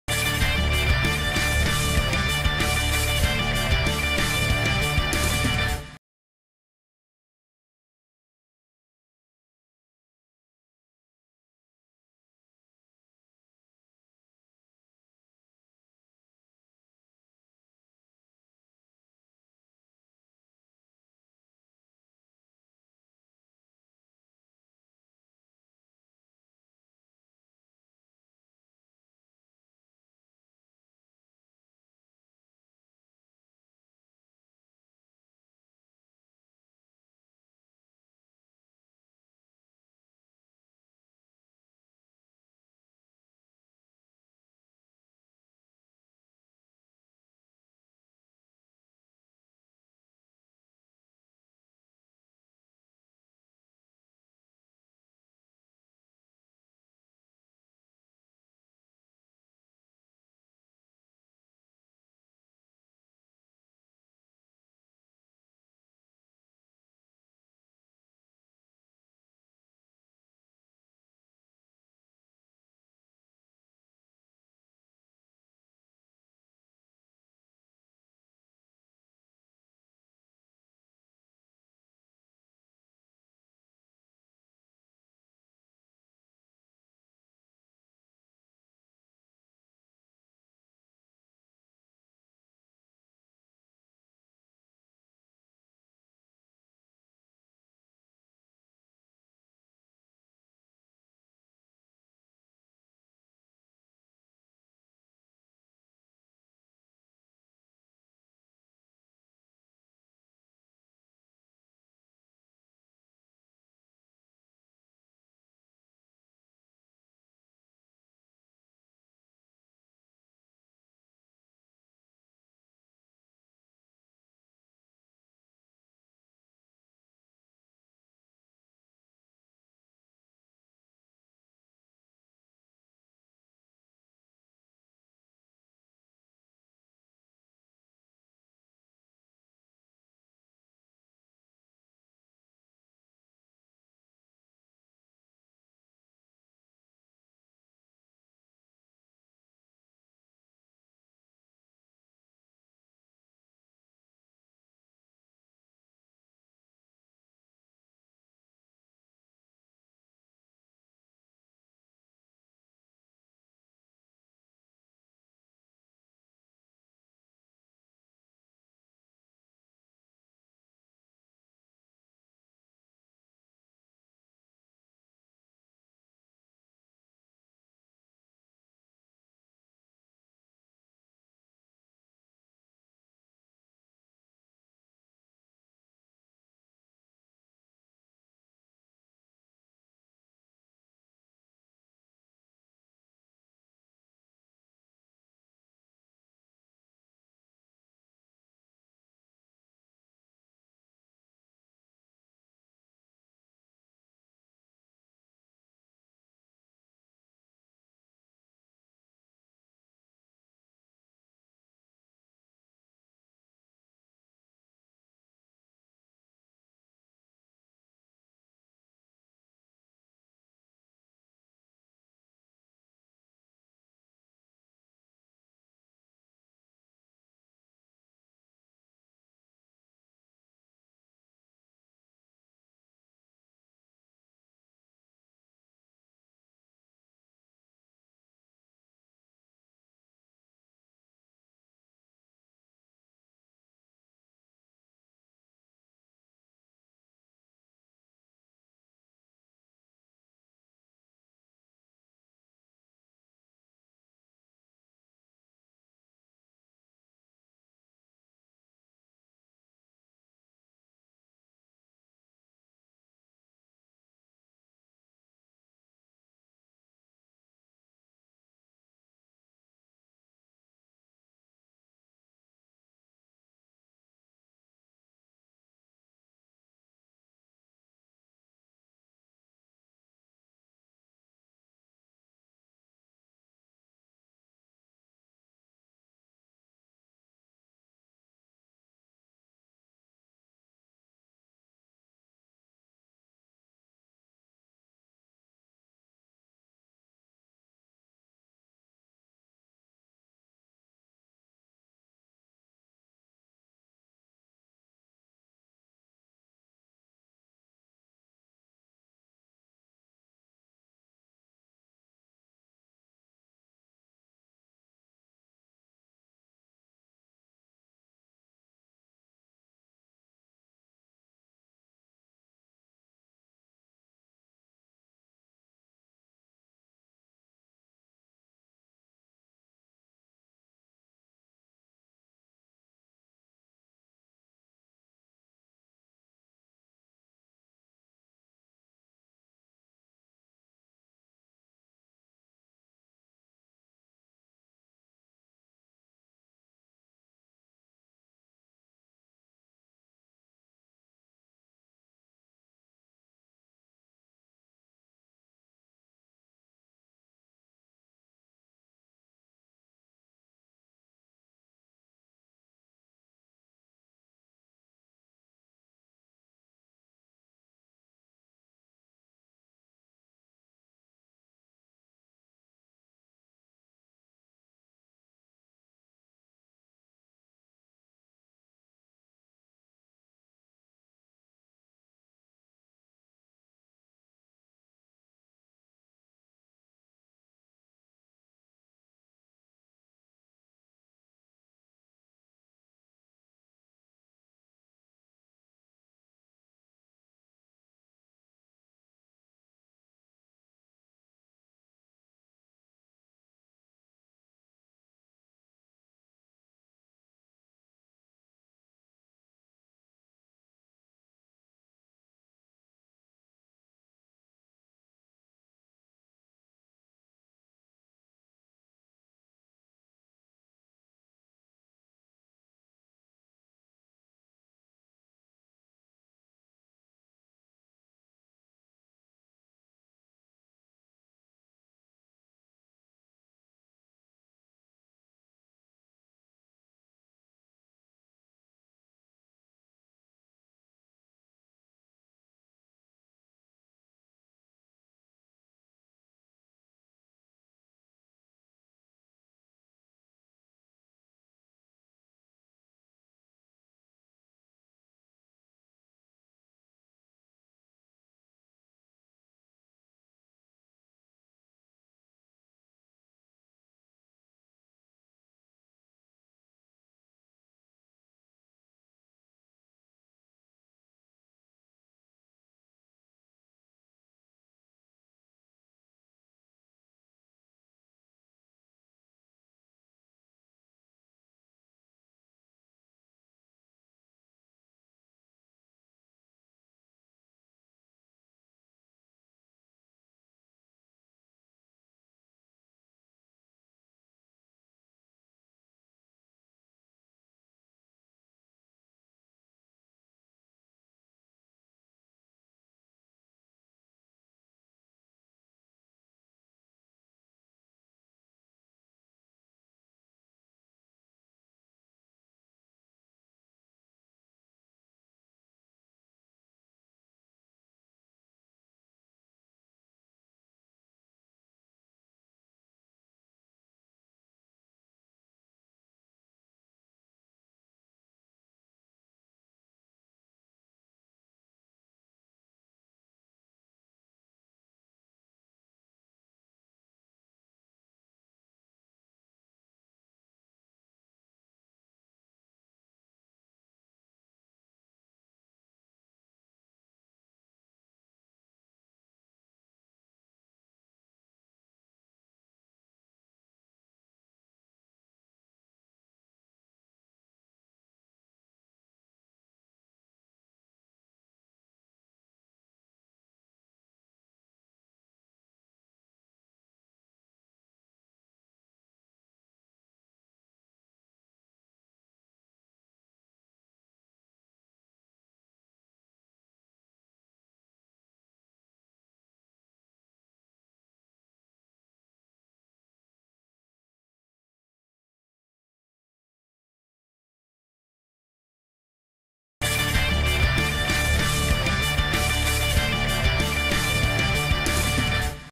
VIVIAN HOO/LIM CHIEW SIEN VS BENYAPA AIMSAARD/NUNTAKARN AIMSAARD, HYLO OPEN 2022. STATISTICS MATCH.
Statistics Match :
Hylo Open 2022
Round of 16 Women's Doubles
VIVIAN HOO/LIM CHIEW SIEN (MAS) VS BENYAPA AIMSAARD/NUNTAKARN AIMSAARD (THA)